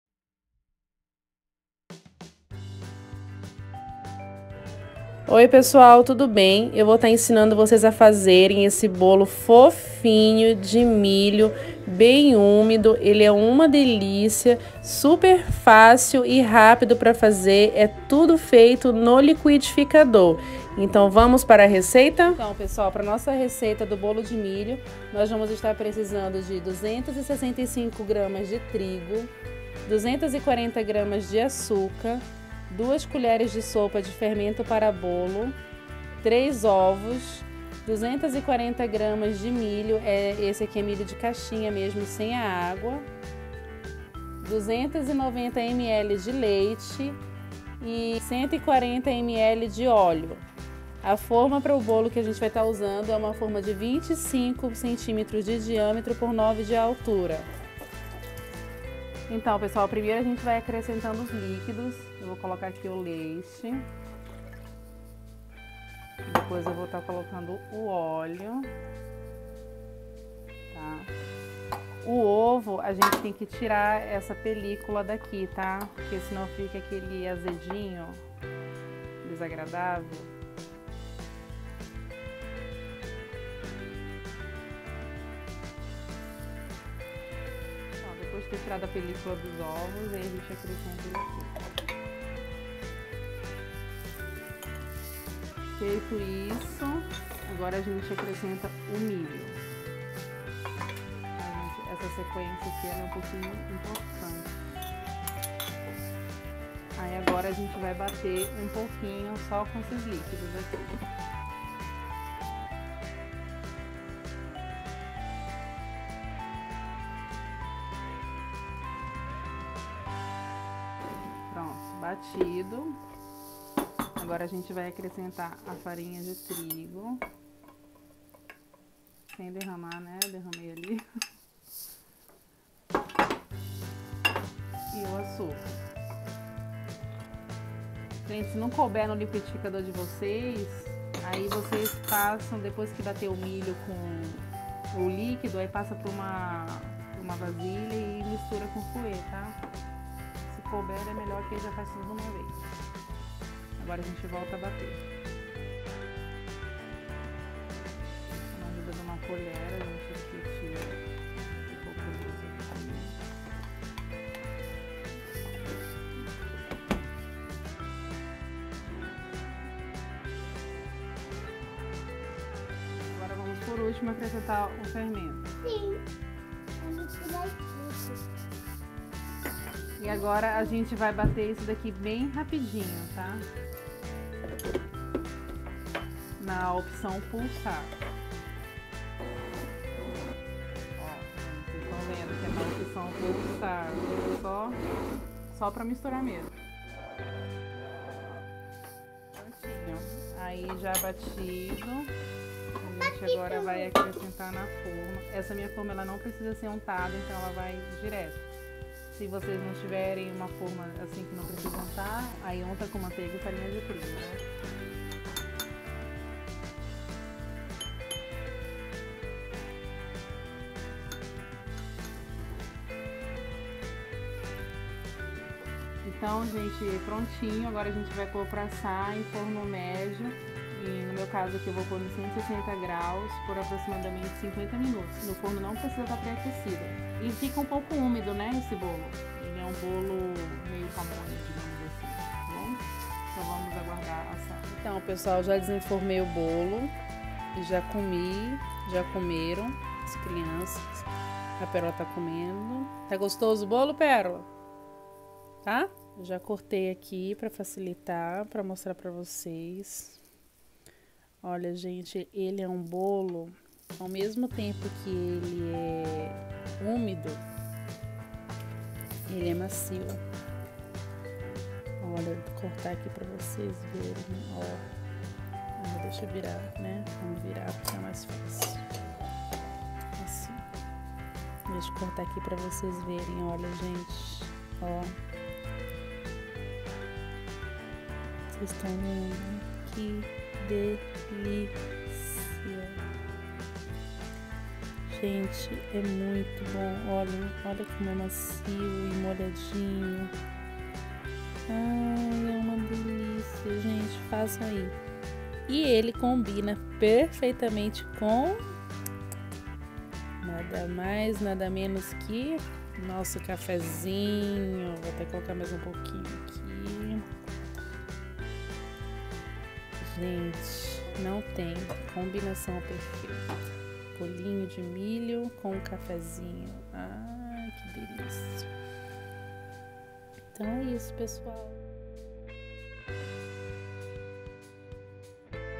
Bolo de milho 🌽 macio e úmido ( rápido e fácil )
Esse bolo de milho 🌽 é macio e bem úmido.
Uma delicia.

Para a receita vamos precisar de:

265 gramas de farinha de trigo
240 gramas de açúcar
2 colher de sopa de fermento ( para bolo)
3 ovos 🥚
240 gramas de milho usei milho de caixinha
290 ml de leite 🥛
140 ml de óleo

Forma usamos uma de 25 cm de diâmetro por 9 de altura